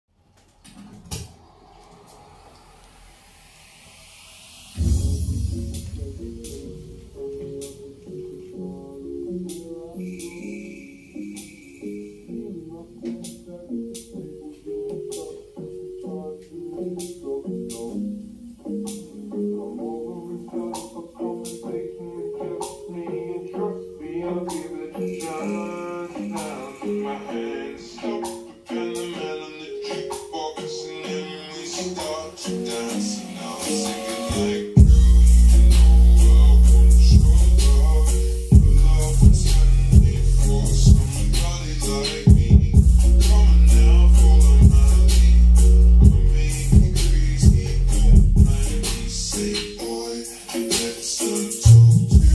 CANDLE TEST !!🔥 JBL PARTYBOX 100 OR CANDEL 🔥🔊 can you gue$$ ?
blutooth - samsung m03s
divice - jbl partybox 100

WHO TALD JBL HAVE NO BASS ?/ [ JBL PARTYBOX 100 NORMAL BASS TEST IN 50 VOLLUME [ BASS BOOST]

."JBL B/T 85 TV ツ community"